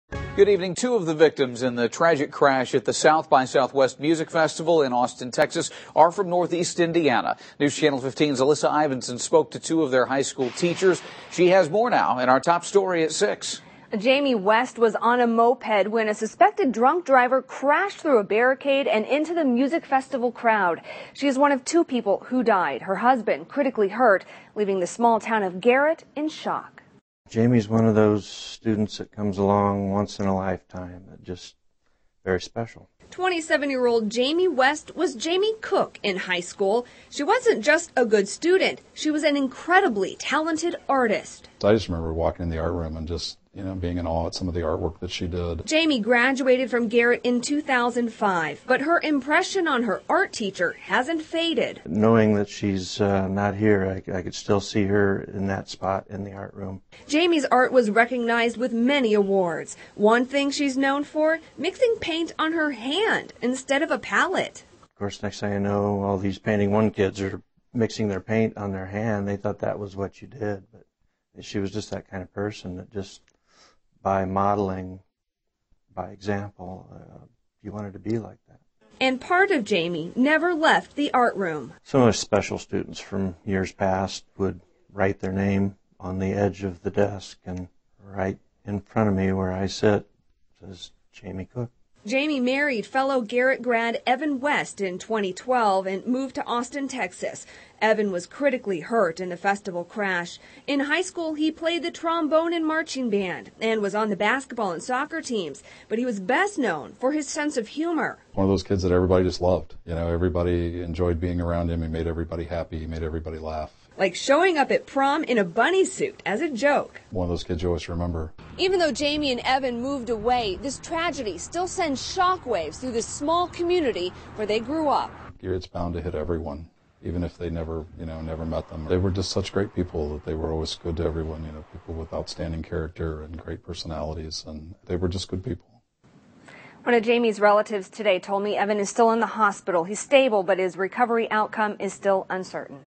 Two victims of Texas music festival crash lived in Garrett
Jamie West died in the tragic crash. Her husband of nearly two years Evan was seriously hurt.